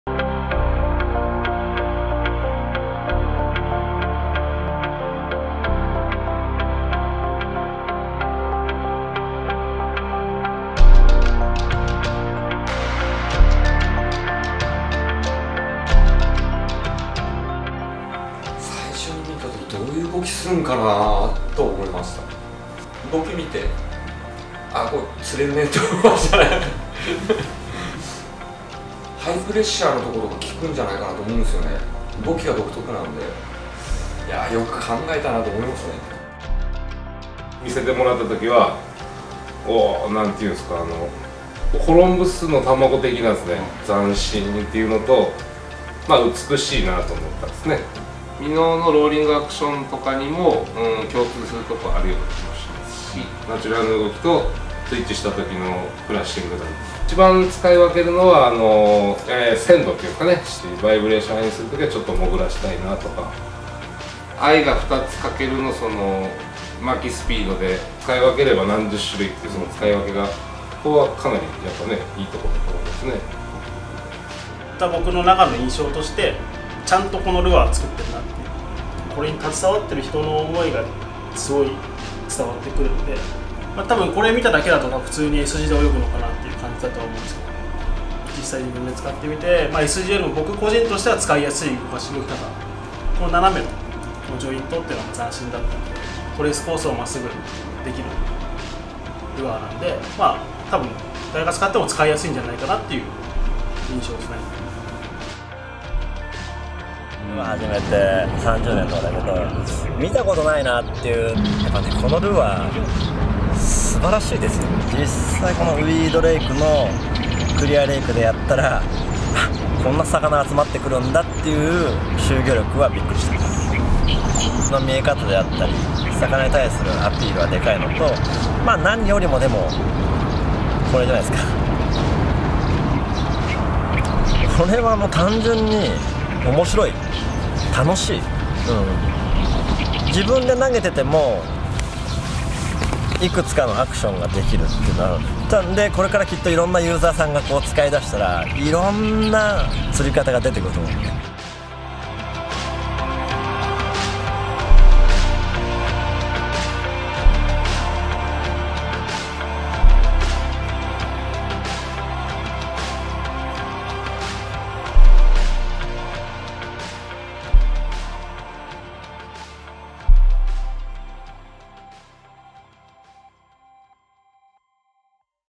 "The Story of Davinci" Chapter01:The ANGLE
ダヴィンチについて、それぞれの想い。

今回は、アングラーからの「想い」。

■Lure
【Davinci】
PAT.P Slanting Joint Lure.

【ダヴィンチ】
特許出願の独自角度ジョイント構造で「リアルな動き」を実現したルアーです。
2つのアイで異なるアクション・レンジを攻める事が出来るルアーになっています。
詳細情報はWEBサイトまで。

■Brand
ELEMENTS®

■Movie
Production：RiGBAT

■Special Thanks

　・KENTARO NAKAMURA
　・KOSEI SHIMOGAMA

　・SHOHEI TOKUNAGA

Masato Takagi